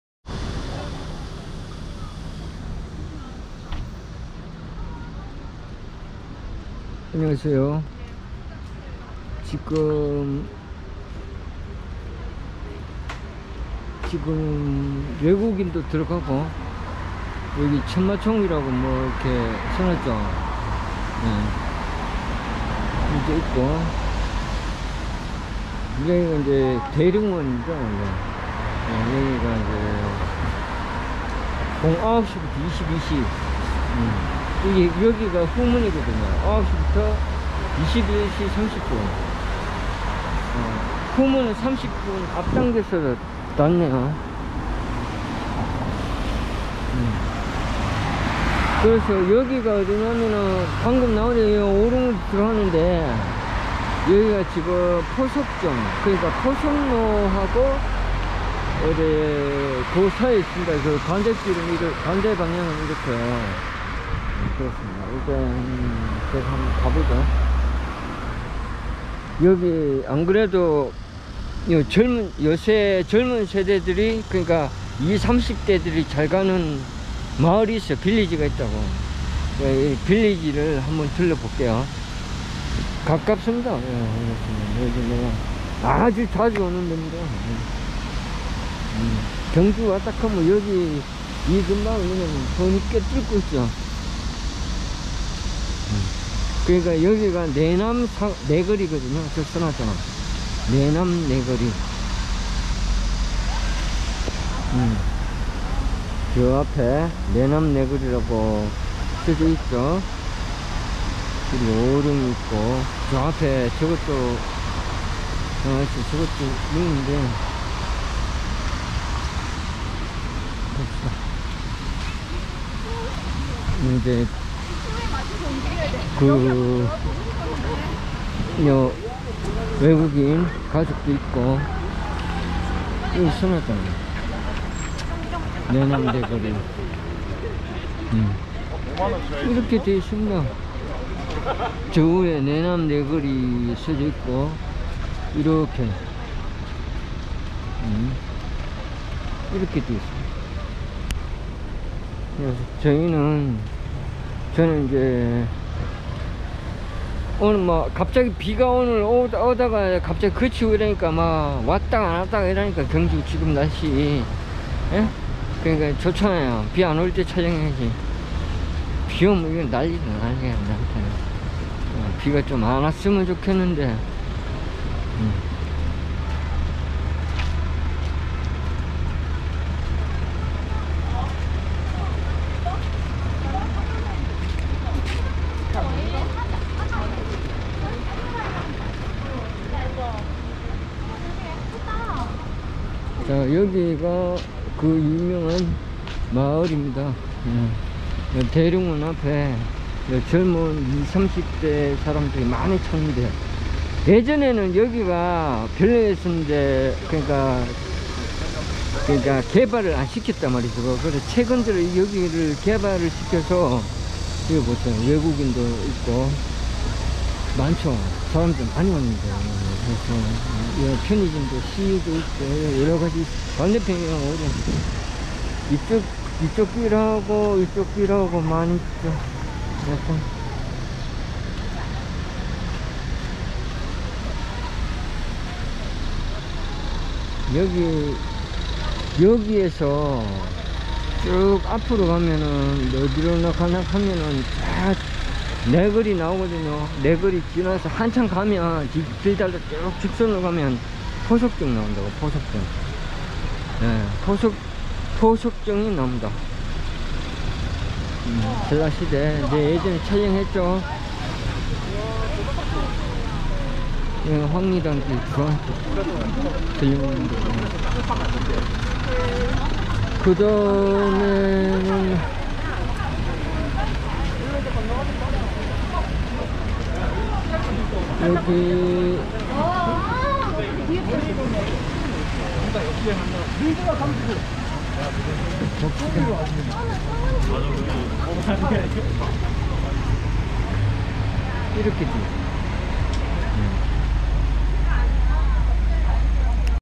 경주답사 Vlog(대릉원)
내남네거리 주변은 황남동 이라 말한다. 이곳은 매일 사람들로 북적이는 경주 명소중 하나이다.
대릉원 주변으로는 월성으로 가는길목과 보문단지 가는길이 동편 쪽으로(대릉원 기준) 가면되고 서편으로는 경주터미널로 갈수있는, 경계지점이라 위치가  기준점을 잡고있어 20-30대 사람들이 꾸준히 찿는 지역중 한곳이다.경주는 다 아는 대로 야외박물관 이라 불리운다. 대릉원과 그주변은 현재 까지 꾸준하게 건축물이 생기는것을 목격하게 된다.